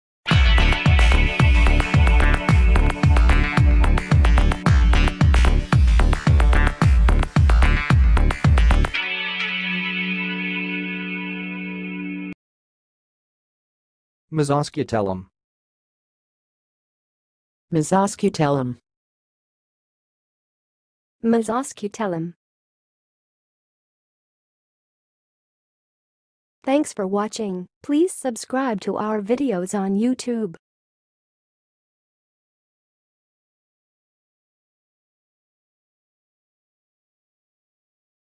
How to say "mesoscutellum"! (High Quality Voices)
Watch in this video how to say and pronounce "mesoscutellum"!